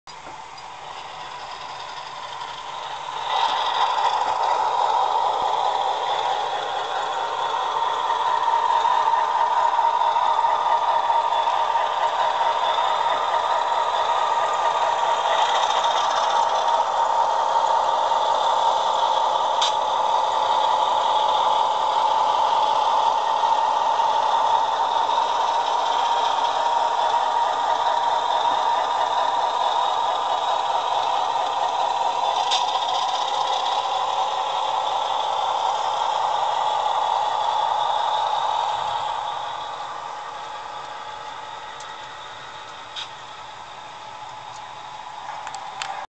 Utah Railway Alco going up hill
A friend of mine built this locomotive from an Atlas RSD 4/5 which I installed a Soundtraxx Tsunami Alco 255 decoder in.  Lights are Golden Glow LEDs with resistors powered off the 12V connections, speaker is a 3/4" round mounted facing up in the cab, cab windows were opened up to let sound out.